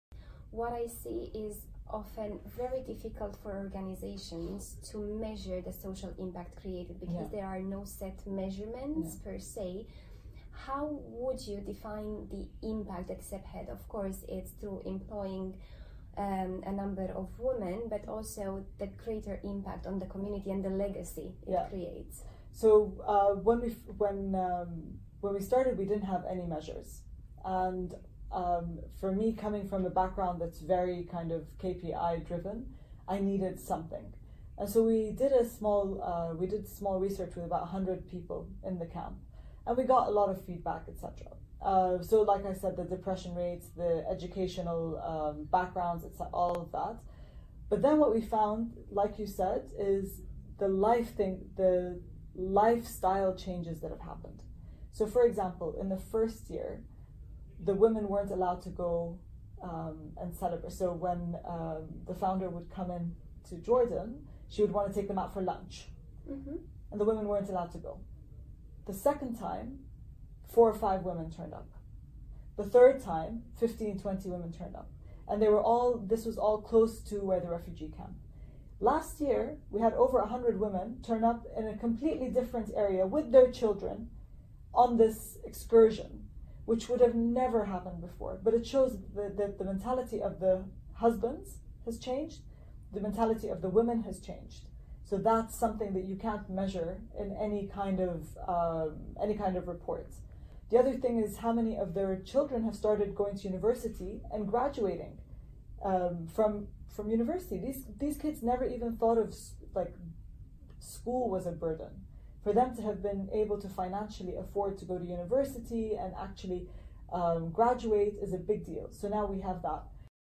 Helen Al Uzaizi: Measuring Social Impact
Measuring impact is essential for any social enterprise in order to track the effectiveness of the organization's main aim. Impact metrics are also a vital tool enabling us to understand the progress towards achieving the SDGs. It is however more complex to calculate impact than financial returns.